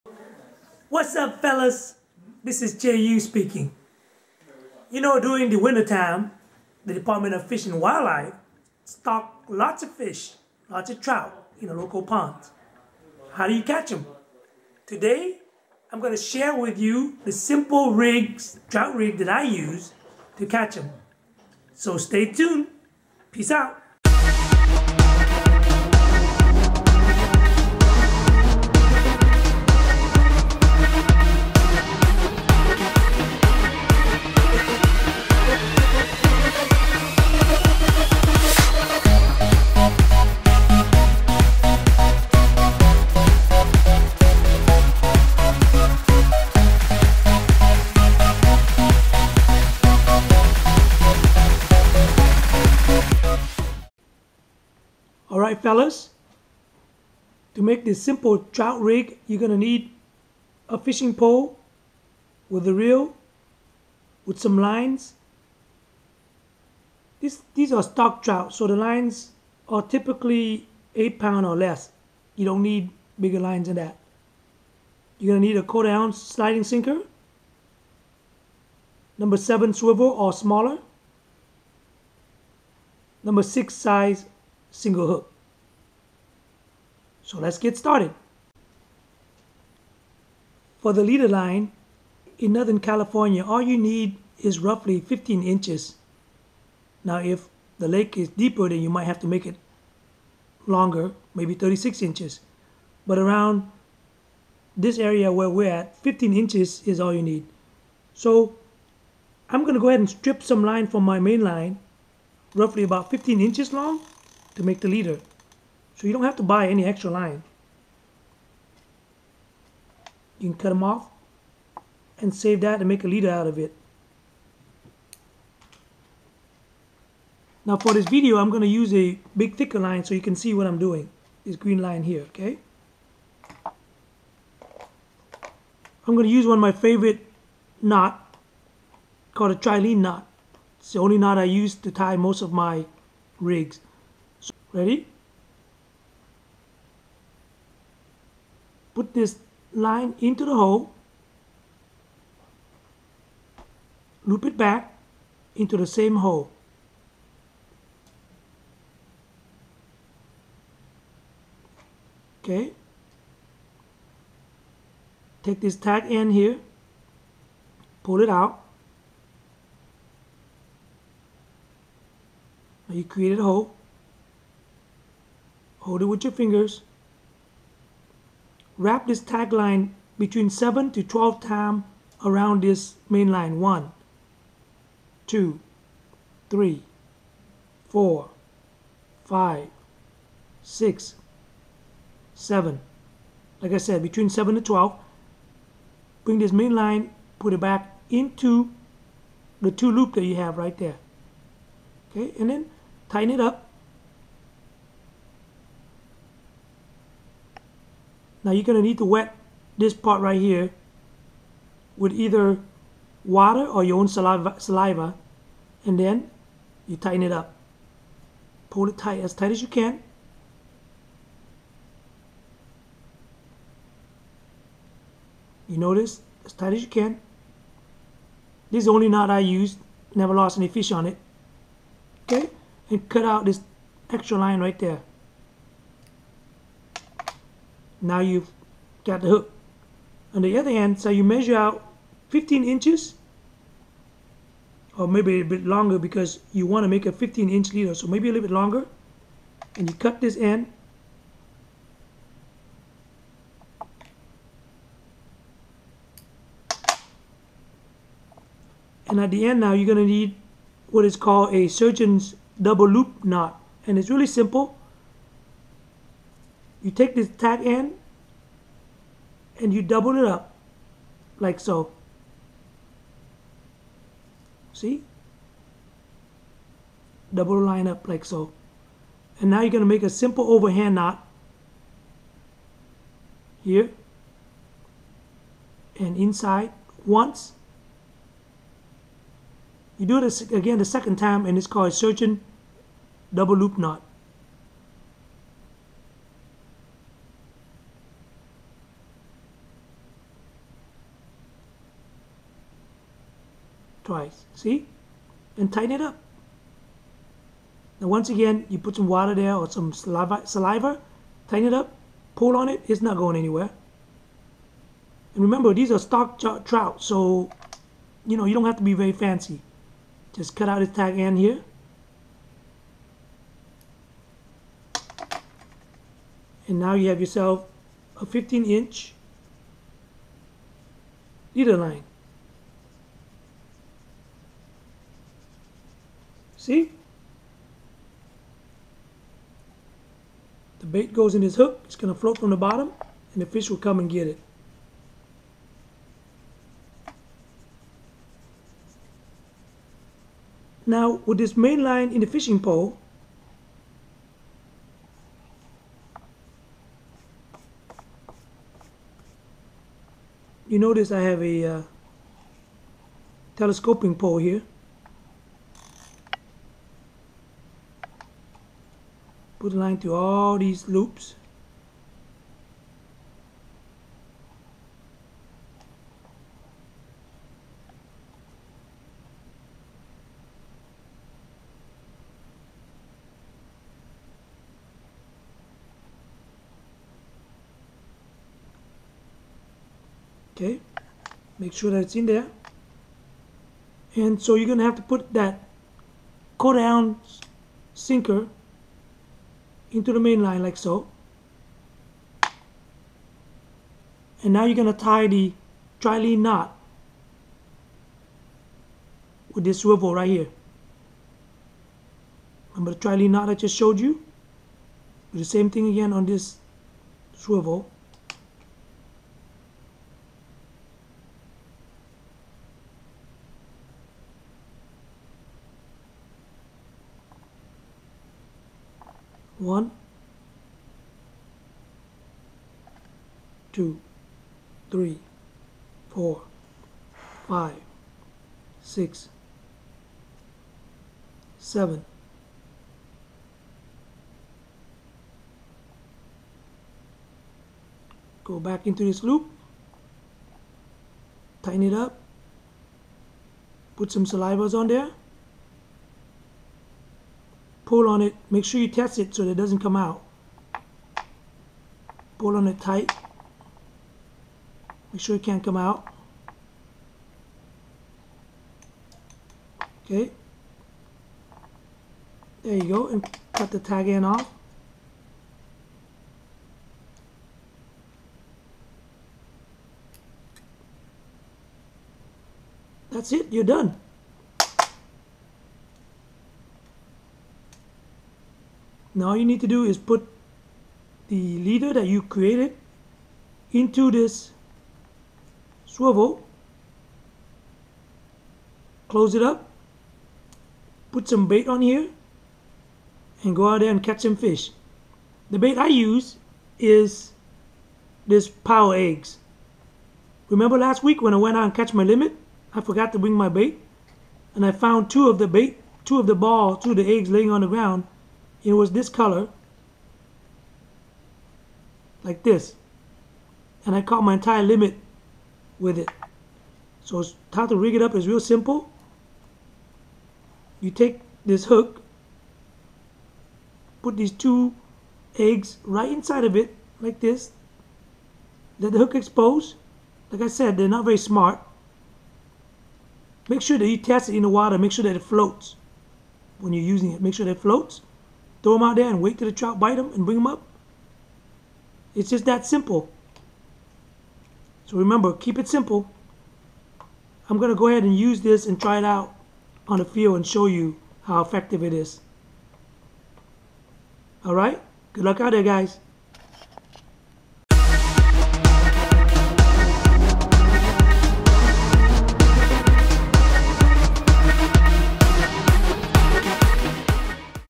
Simple Trout Rig
Catching stocked trout in Northern California is super easy with this simple trout rig.  The secret is finding out where the trout are located.  In this particular week, this simple trout rig was tested and verified at the Elk Grove Pond.  To see this Simple Trout Rig in field testing, view this

Instagram & Twitter: @JUNationFishing